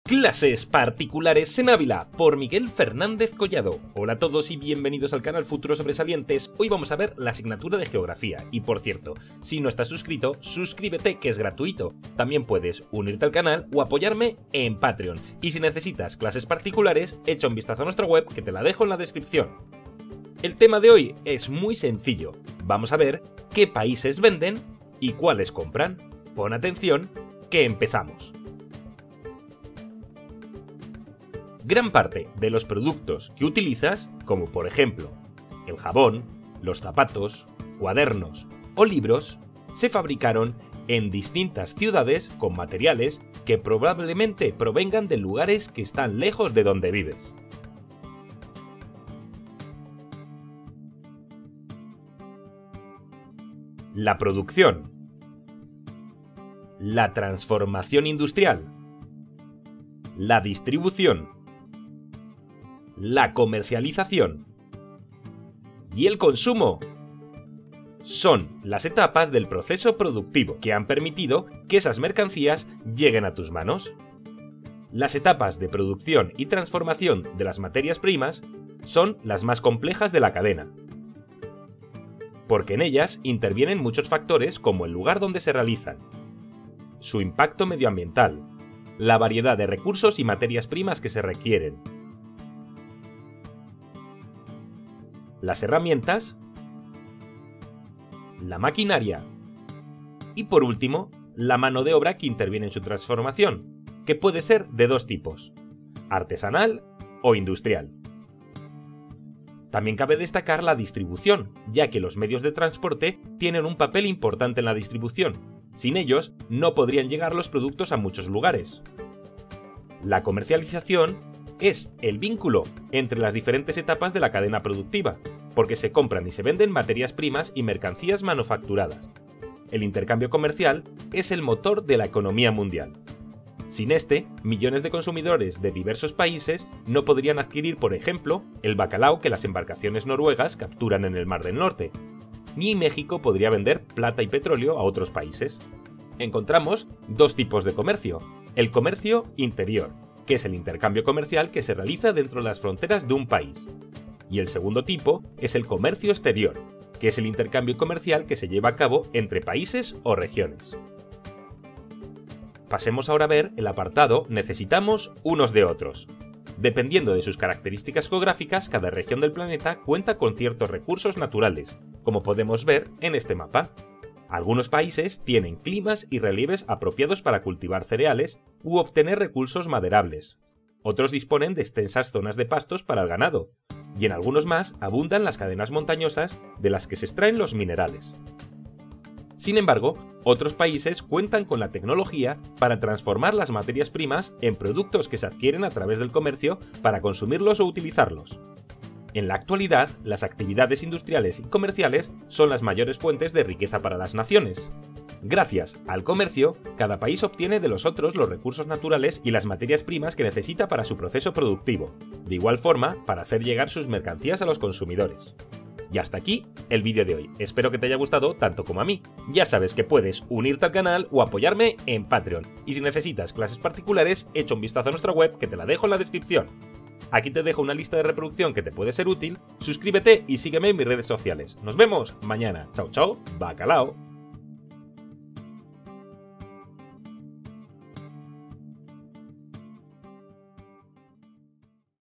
🤑 Qué PAÍSES VENDEN y cuales COMPRAN | Geografía sexto grado
🤑 Hoy vamos a explicar fácilmente qué países venden y cuáles compran de geografía para sexto grado

Para alumnos de 6º grado de geografía

COMPRA en Amazon con este enlace